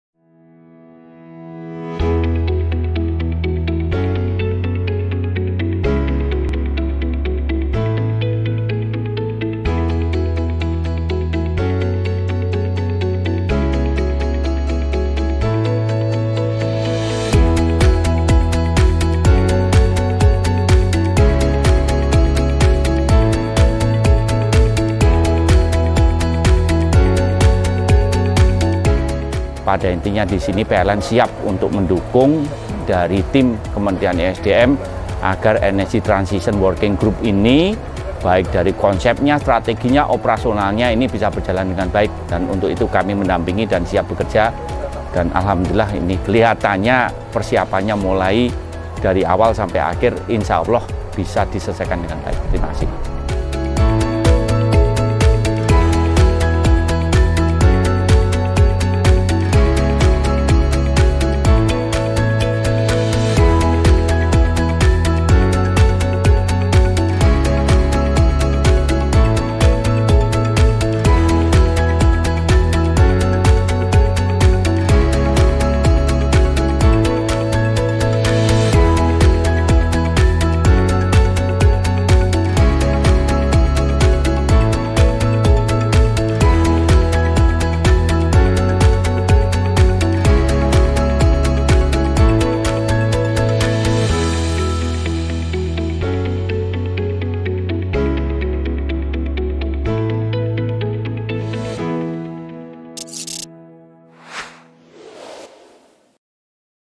PLN Dukung G20 Energy Transition Working Group di Yogyakarta
Electrizen, sebagai rangkaian Presidensi G20 yang akan digelar Oktober nanti, PLN dukung penuh penyelenggaraan Energy Transition Working Group (ETWG) yang diselenggarakan Kementerian ESDM di Daerah Istimewa Yograkarta pada 24—25 Maret 2022 mendatang.

Selain itu, dalam acara tersebut PLN juga akan melakukan Penandatanganan Power Purchase Agreement (PPA) pembangkit energi baru terbarukan (EBT), penandatanganan MoU Green Financing dan penandatanganan pembelian sertifikat EBT ( renewable energy certificate /REC).

" Energy transition ini adalah hot topic, kami sedang merancang bagaimana generasi masa mendatang mempunyai masa depan lebih baik dari masa saat ini, caranya adalah dengan mengurangi emisi," imbuh Darmawan Prasojo, Direktur Utama PLN.

PLN